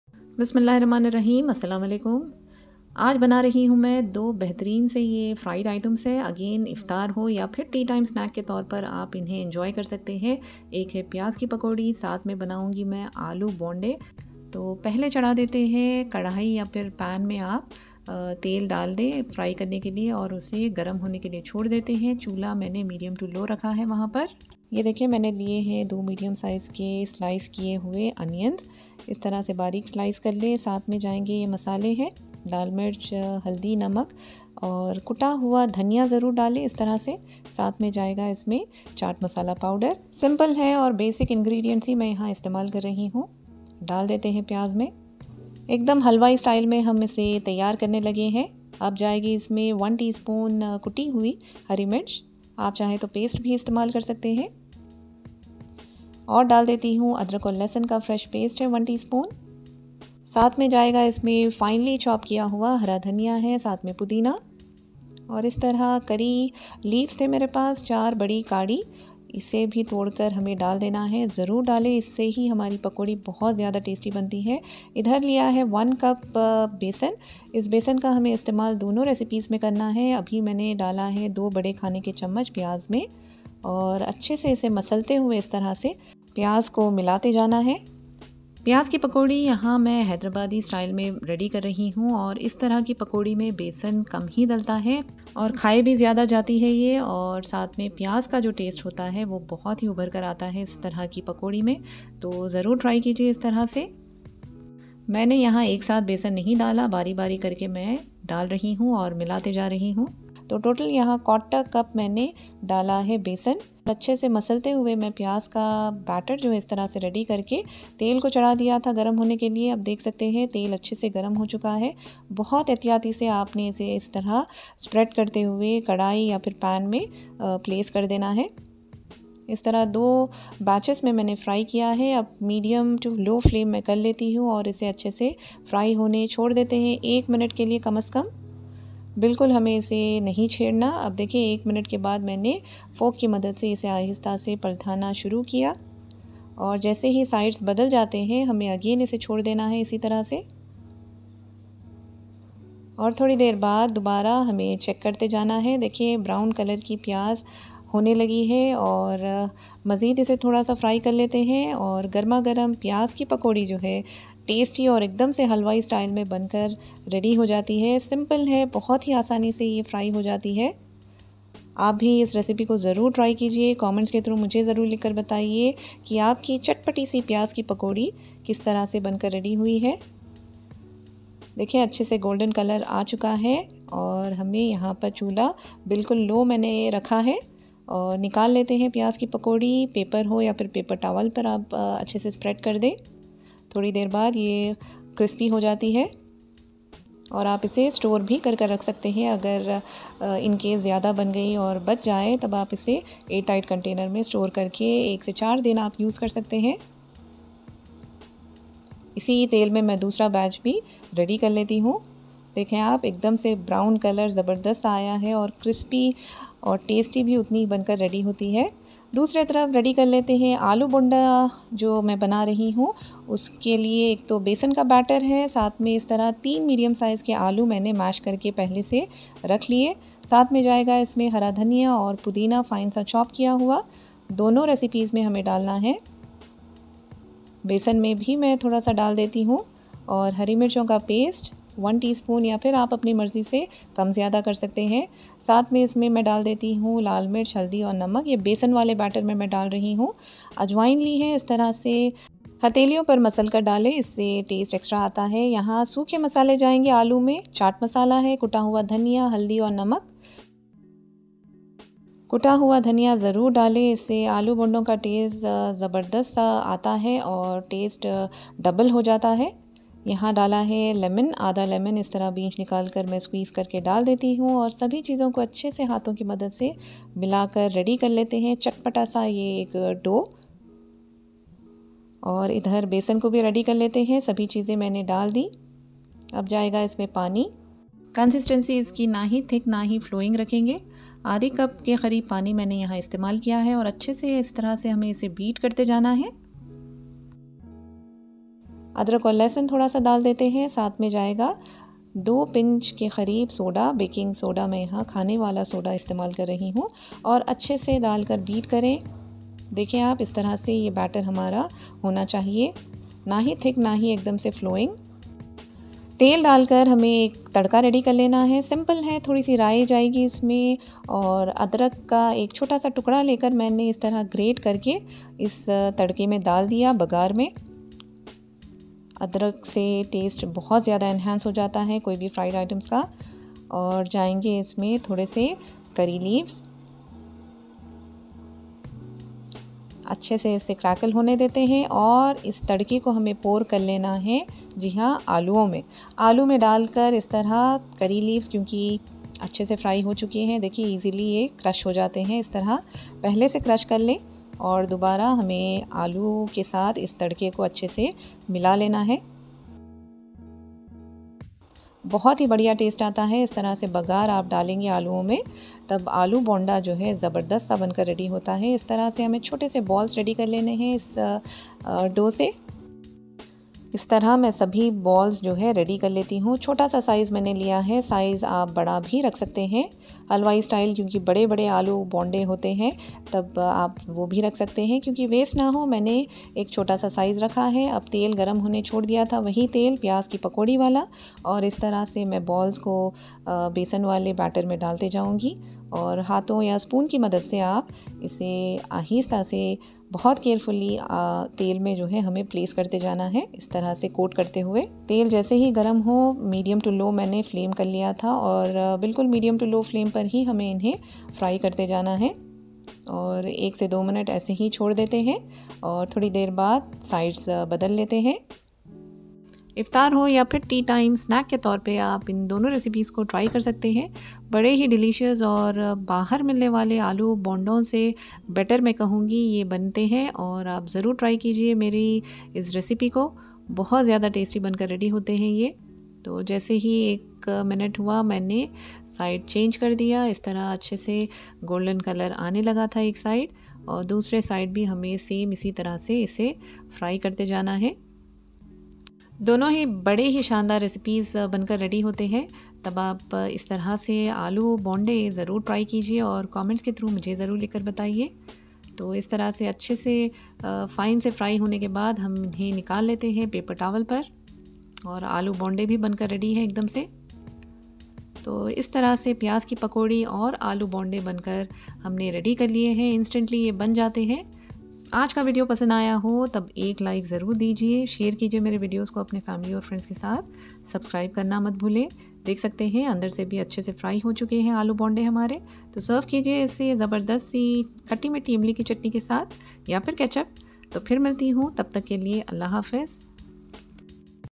AALOO BONDA & PYAAZ KI PAKODI (Halwaei style special)
AALOO BONDA|Halwaei style recipe 😀
Requirements;
3 medium size potatoes boiled, peeled and mashed
finely chopped fresh herbs
1 tspn green chillies coarsely grinded
1 tbspn chat masala
1 tspn cuta hua dhania
1/2 tspn turmeric powder
1 tspn salt

for Tadka;
2 tbspn oil
1/4 tspn rai or mustard seeds
1 inch grated ginger

Add into the potatoes and make equal size ball and keep aside

Besan or Gramflour Batter:
3/4 cup Besan or Gramflour
1 tspn salt
1 tspn red chilli powder or to taste
1/2 tspn turmeric powder
few finely chopped fresh herbs
green chillies chopped or paste 1/2 tspn
1/2 tspn ginger garlic paste
2 pinch baking soda
water accordingly until the desired consistency of the batter

beat well while mixing
for frying heat oil in a pan or kadai
Dip potato balls into the besan batter & then into the hot oil very carefully,
within mins hot and tasty Halwaei style Aaloo Bonda will be ready, serve hot with ketchup or any tasty chutney.

PYAAZ KI PAKODI(Hyderabadi style):
2 medium size sliced Onions
1 tspn coarsely grinded green chillies
few finely chopped fresh coriander and mint leaves
1 tspn red chilli powder
1 tspn salt
1/2 tspn turmeric powder
1 tspn chat masala powder
1 tbspn cuta hua Dhaniya
handful of fresh curry leaves
1/4 cup Besan or Gramflour
mix everything together and once mixed spread carefully into the hot oil for frying
turn each side after 1-2 mins and once nice golden in colour take out on a paper towel
Crispy & tasty PYAAZ KI PAKODI Hyderabadi Halwaei style is ready 😀